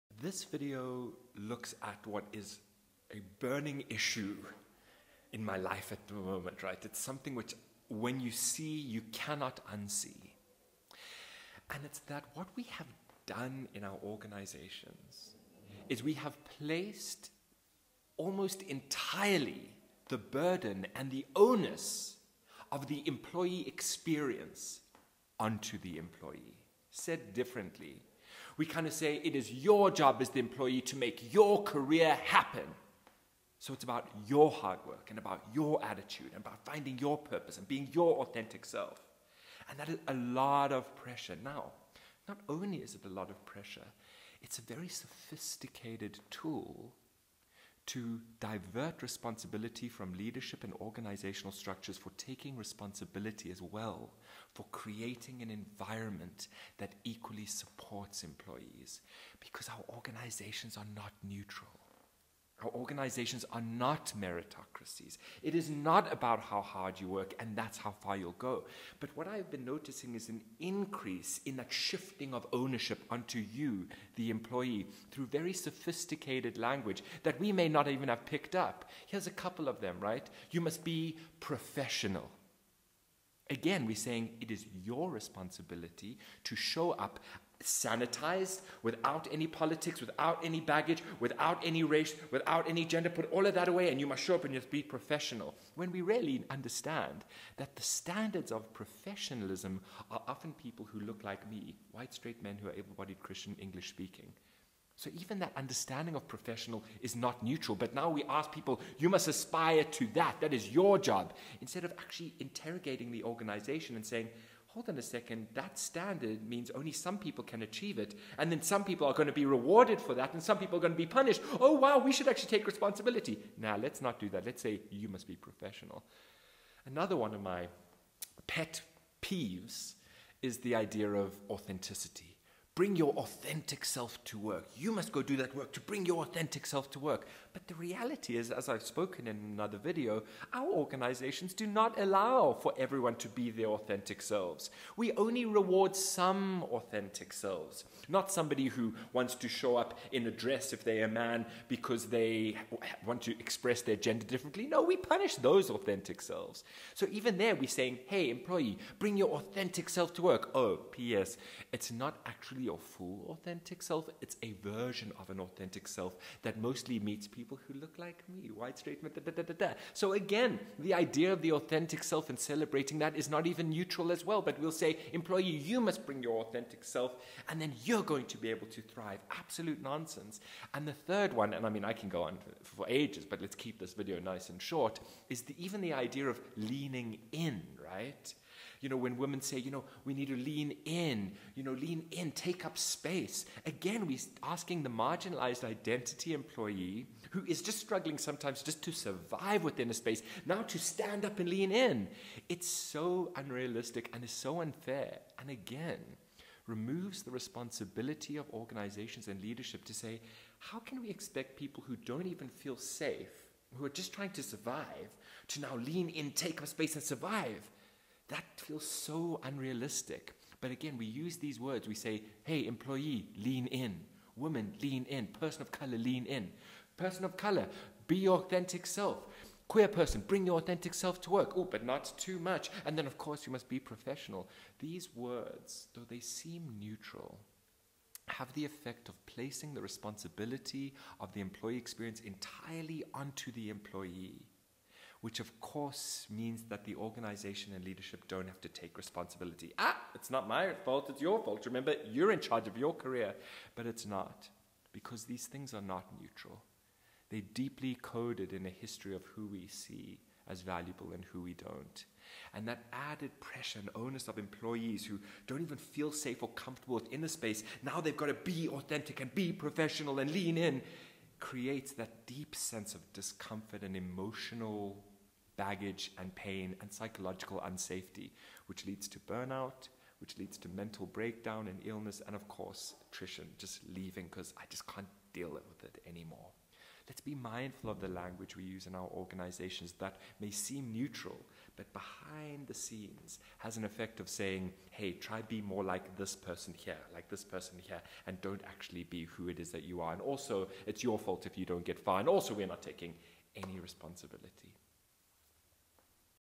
Don't make this mistake when designing the Employee Experience
We have all heard the term 'you must take ownership of your own career'. While yes, there exists an element of truth in this, it disguises a deeper, more insidious truth about our organisations: that we have shifted the onus of the employee experience almost entirely onto the employee. This is a mistake.

Here is why